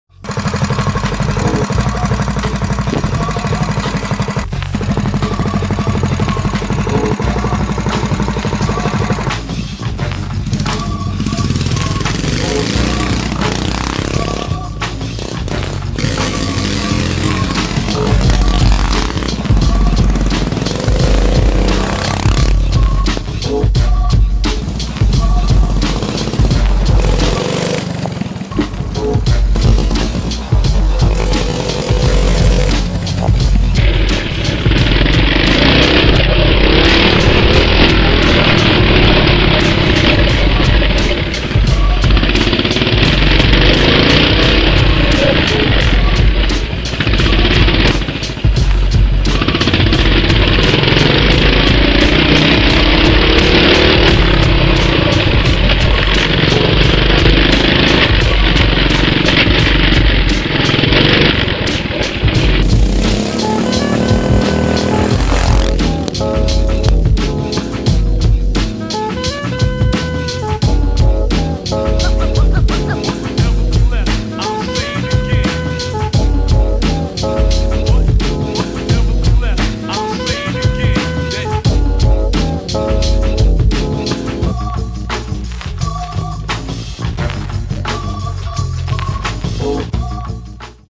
Sprocket Swap (Coleman Ct200U-EX)
Swapped the 50 tooth sprocket for a 60